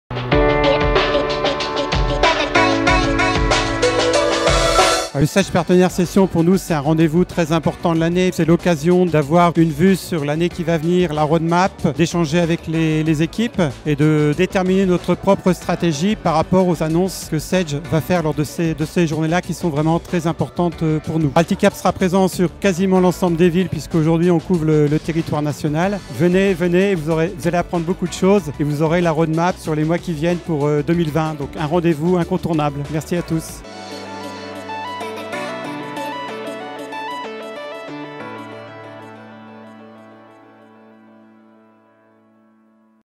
Alticap, présent aux Sage Partner Sessions 2019
Alticap sera présent aux Sage Partner Sessions pour découvrir les nouveautés et l'actualité Sage, et aussi échanger avec tout l'écosystéme partenaire Sage. Découvrez l’interview de Eric Legoff, Directeur Général de Alticap et inscrivez-vous.